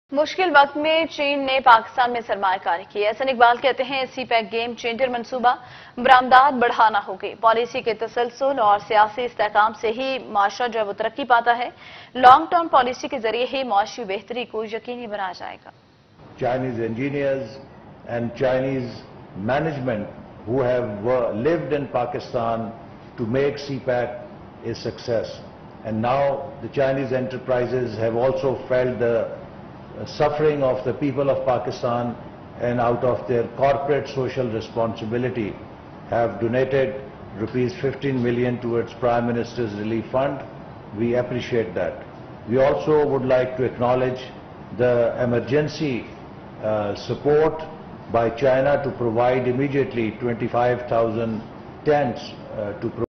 China invested in Pakistan in difficult times, Ahsan Iqbal CPEC game changer project
مشکل وقت میں چین نےپاکستان میں سرمایہ کاری کی،احسن اقبال
سی پیک گیم چینجرمنصوبہ،برآمدات بڑھانا ہونگی